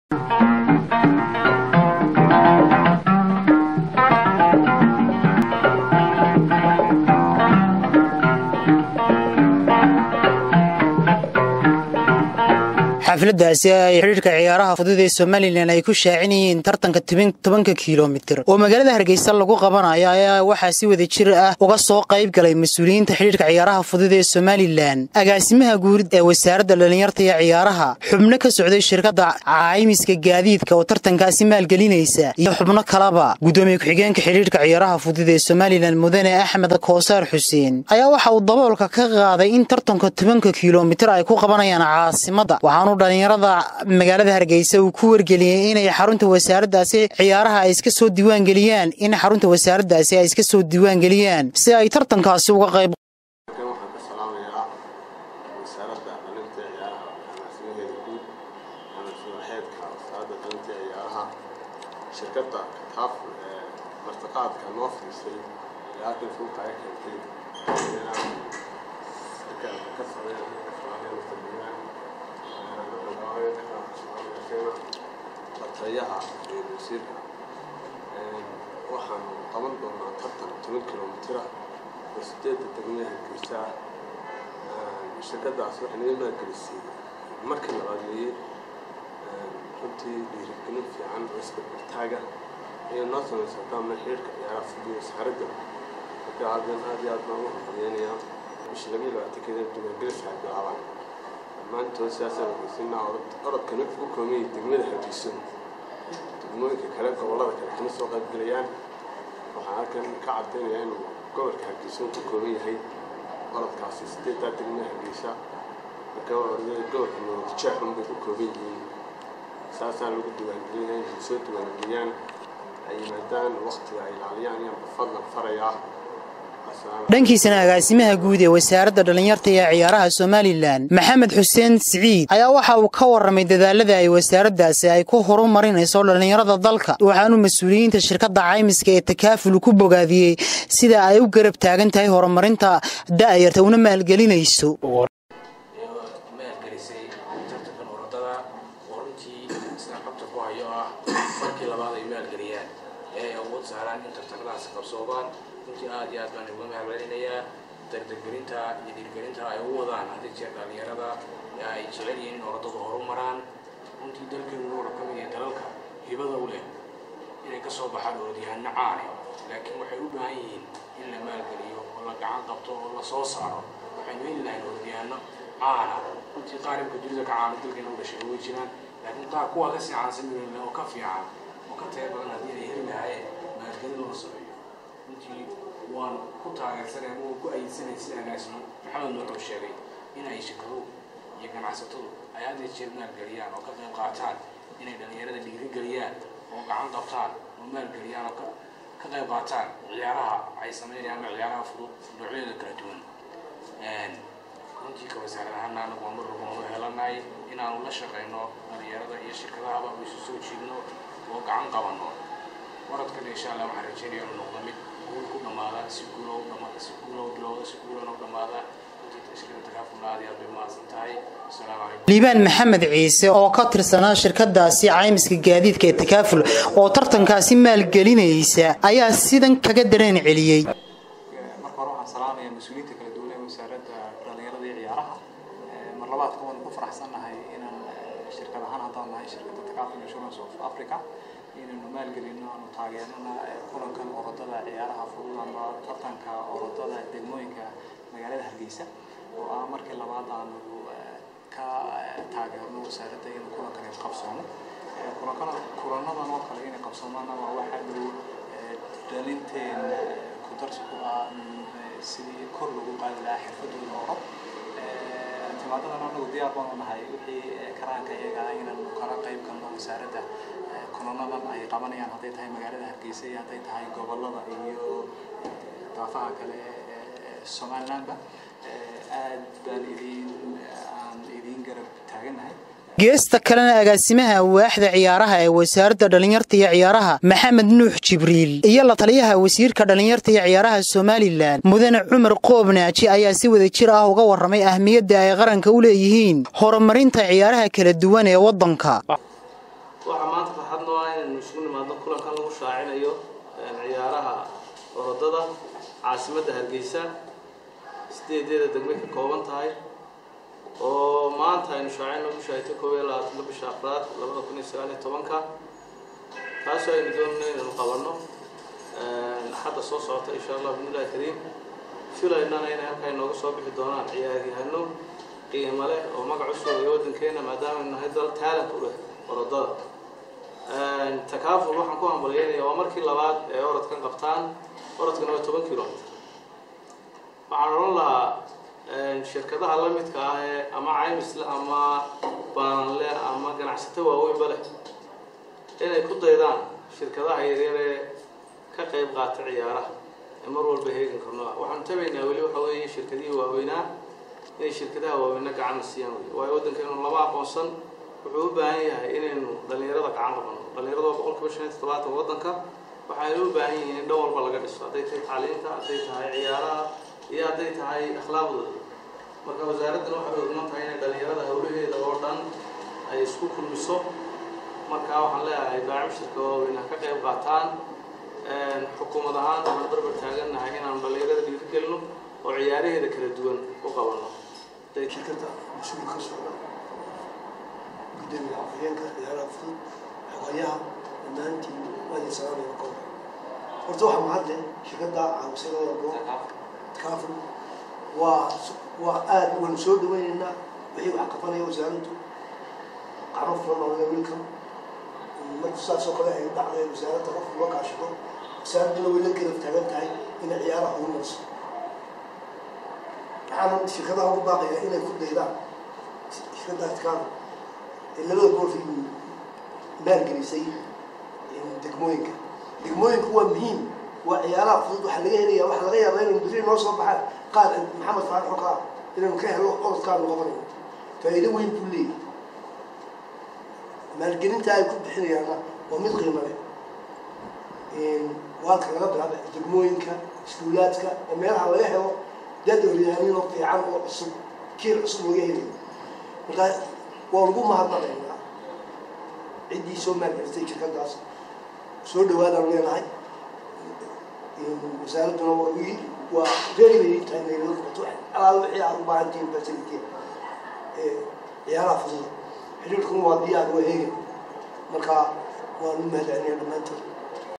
DEGDE XIDHIIDHKA CIYAARAHA FUDUD SOMALILAND AYAA DABOOLKA KA QAADAY IN AY CAASIMADA KU QABANAYAAN MA
XIDHIIDHKA CIYAARAHA FUDUD SOMALILAND AYAA DABOOLKA KA QAADAY IN AY CAASIMADA KU QABANAYAAN TARTANKA ORODKA 10KA KILOMITIR.